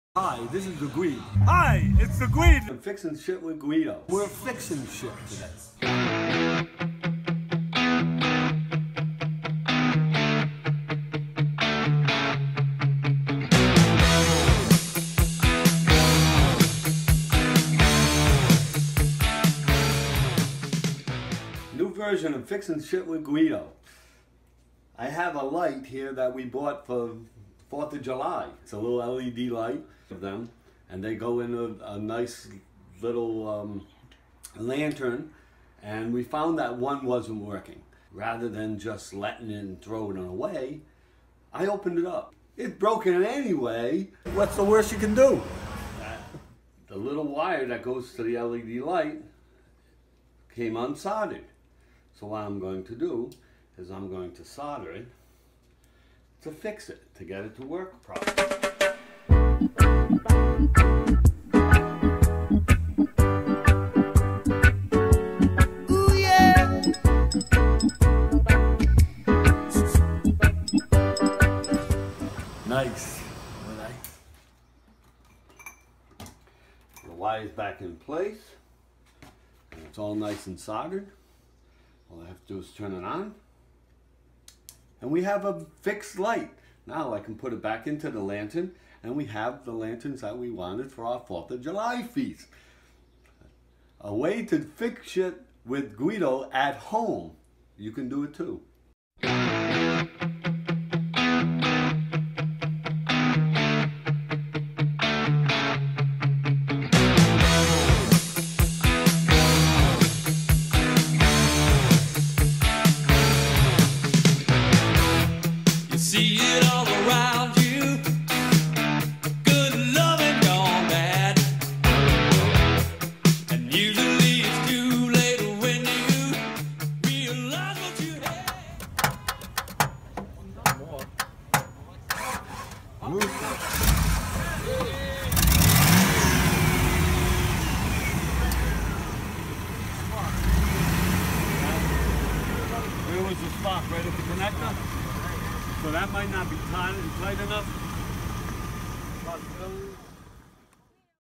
Fixin' Shit with Guido - Home Edition
Guido gives a tip on how to save your summertime holiday celebration.